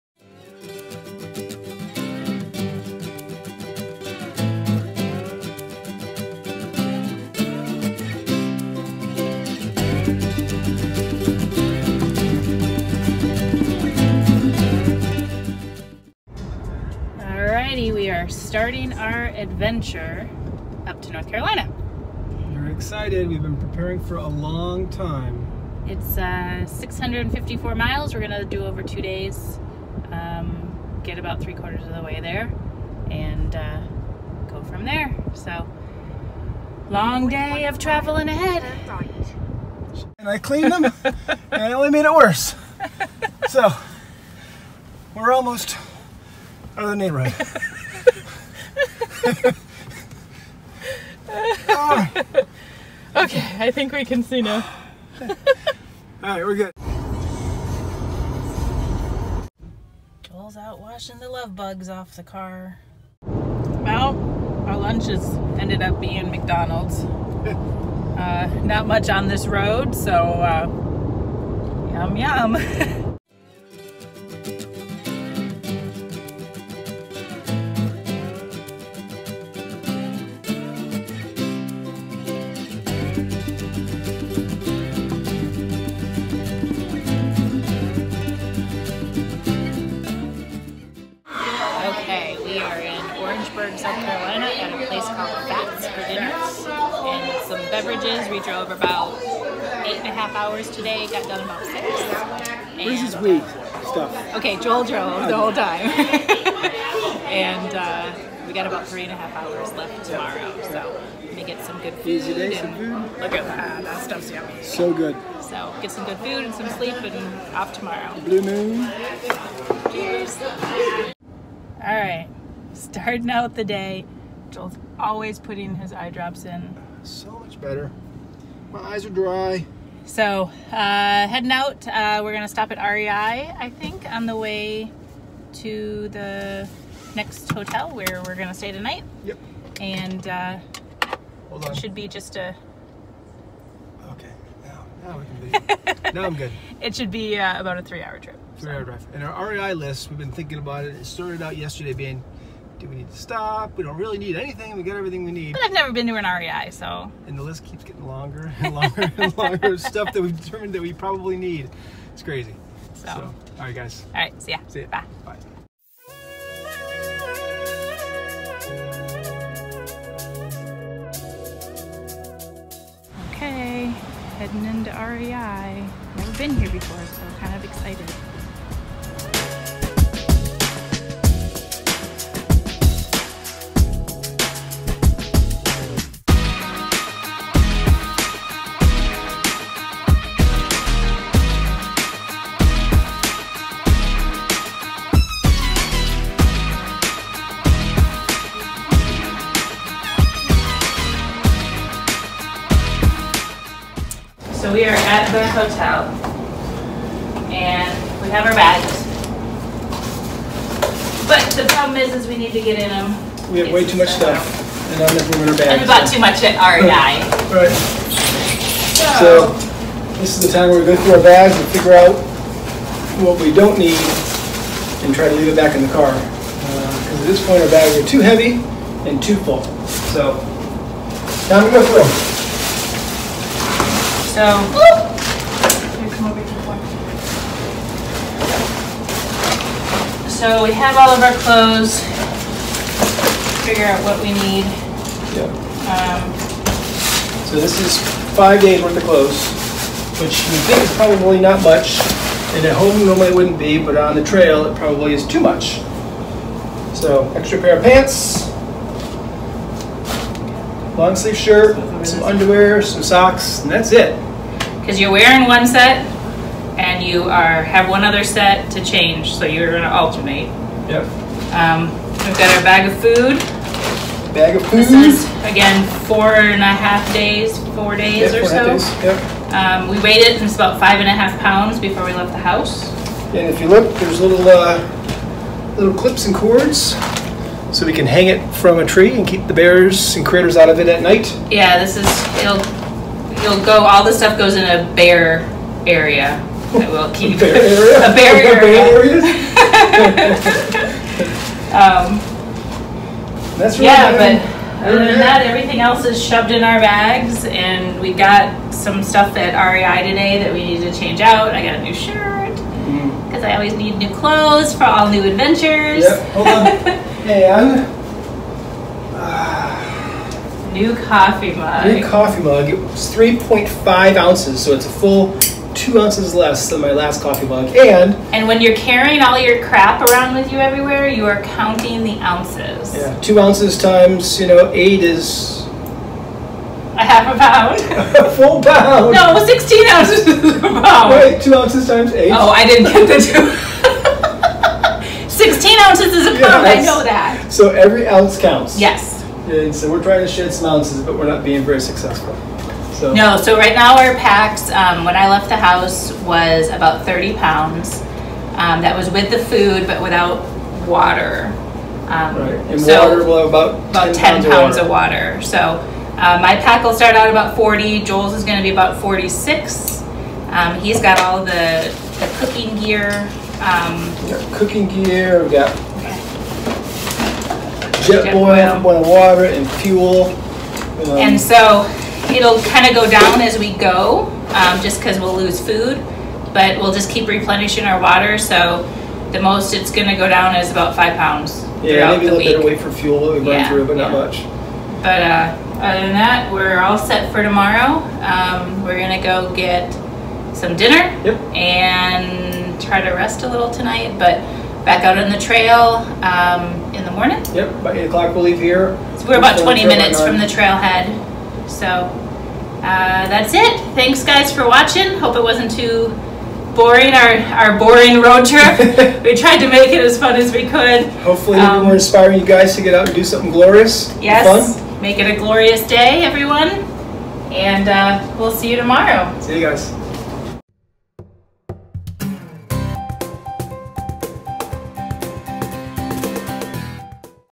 Road Trip to NC
Join us on our 2 day road trip to North Carolina before our hiking trip at Linville Gorge.